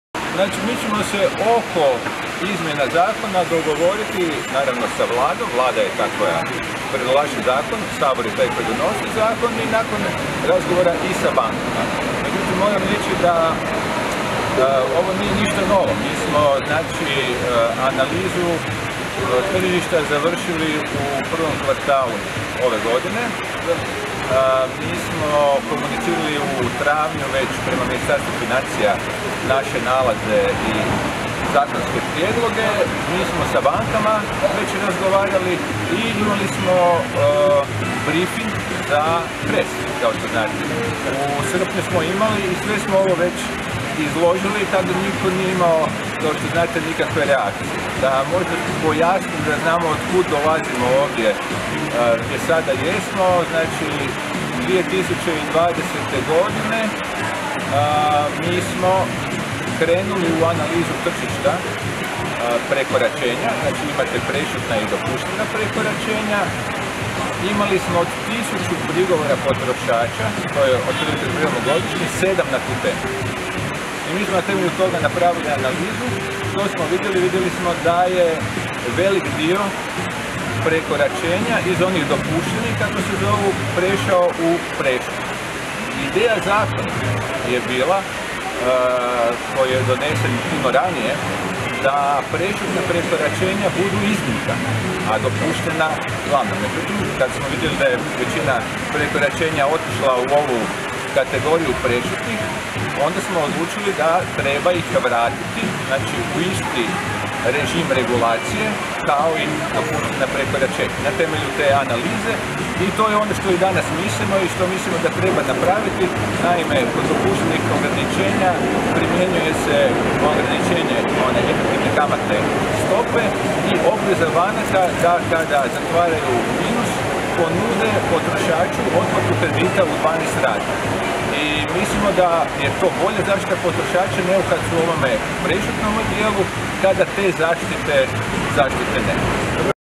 Guverner Hrvatske narodne banke Boris Vujčić o problemu prešutnih minusa građana u bankama
Guverner Hrvatske narodne banke Boris Vujčić o problemu prešutnih minusa građana u bankama (Robert Anić/PIXSELL).